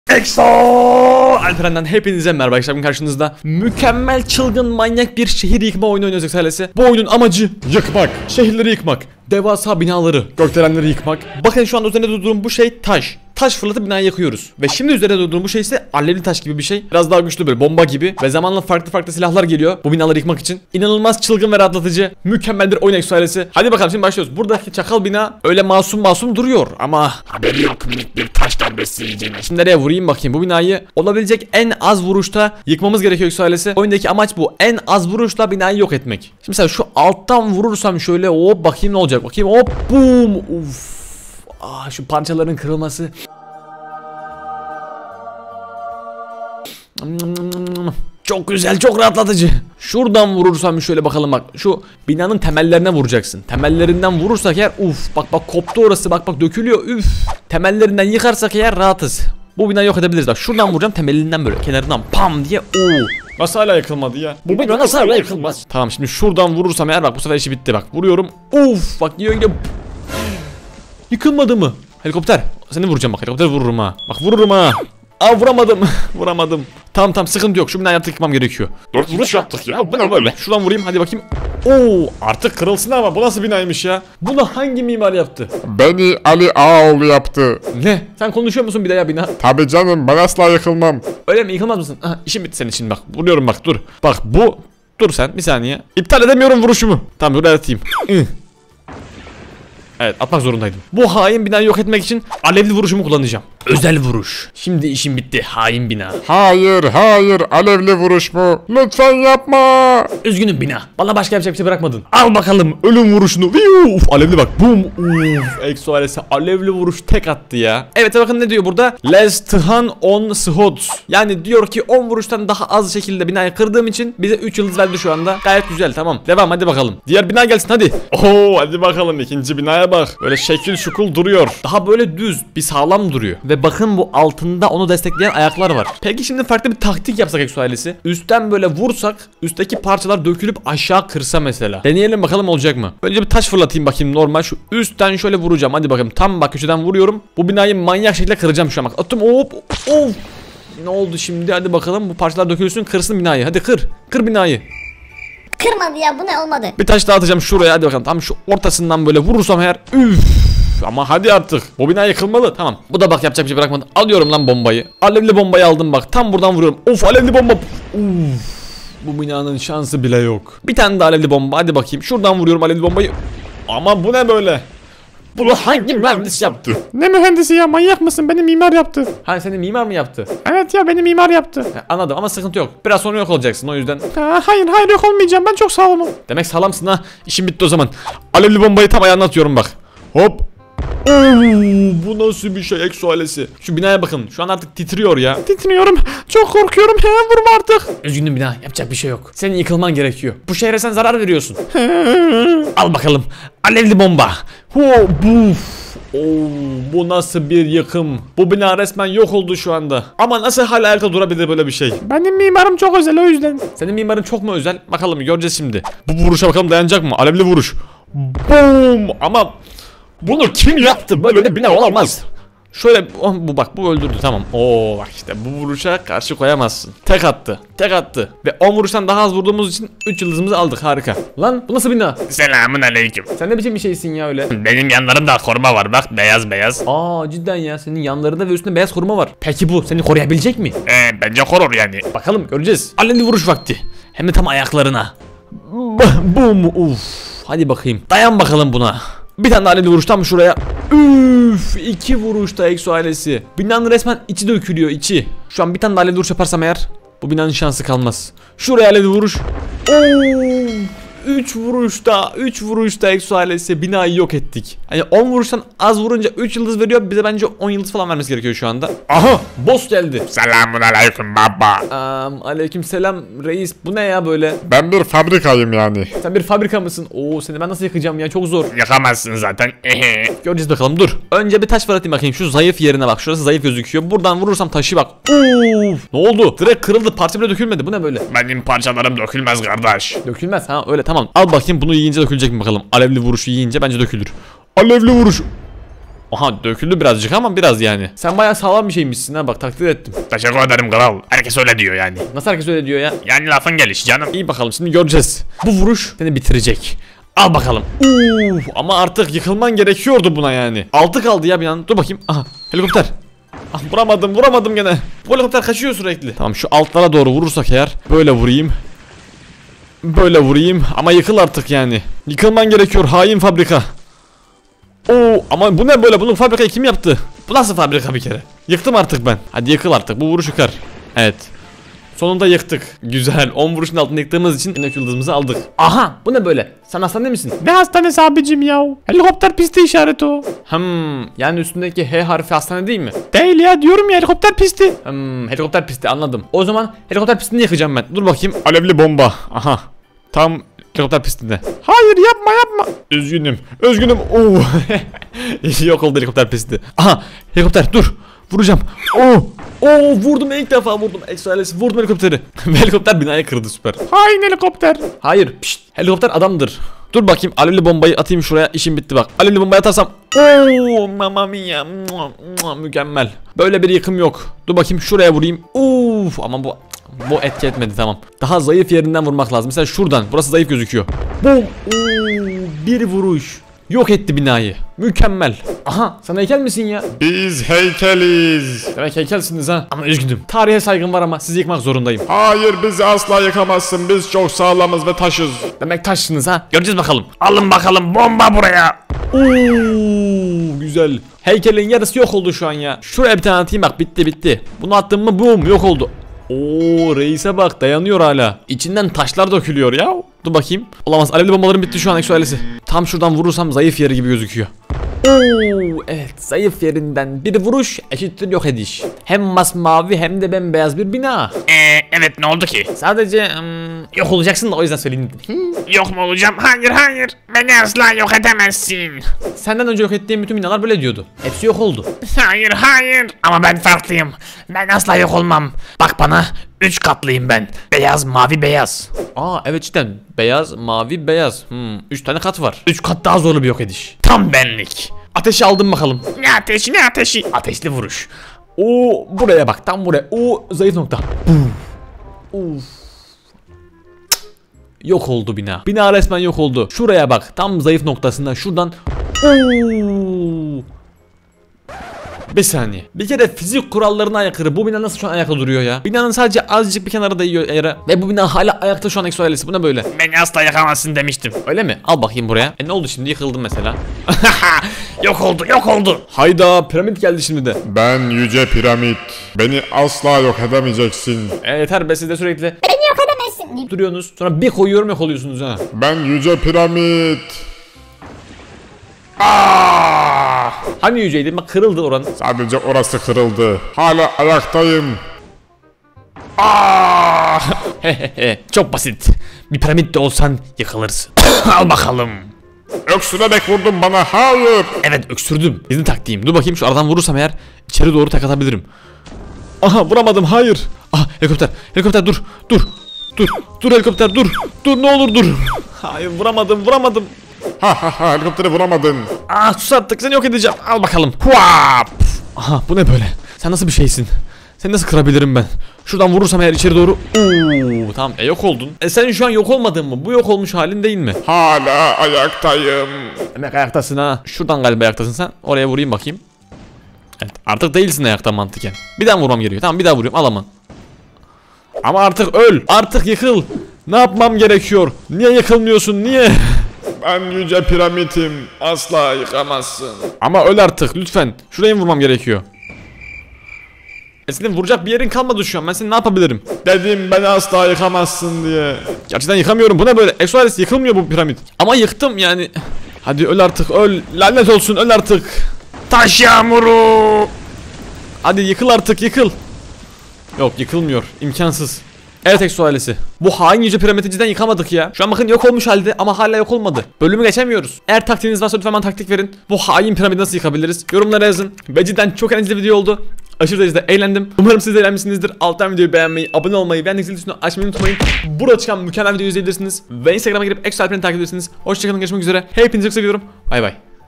EV YIKMA OYUNU 💥
Merhaba XO Ailesi ,  Bugün rahatlatıcı oyun olan aynı zamanda android oyun lardan biri olan ev yıkma oyunu nu oynadım xo alperen bina yıkma oyunu farkıyla sizlere sundum , gerçekten çok komik çok eğlenceli bir video oldu , iyi seyirler.

XO Ailesi nin instagram ını takip et !

Brawl Stars videolarım ile sizleri her gün eğlendiriyorum,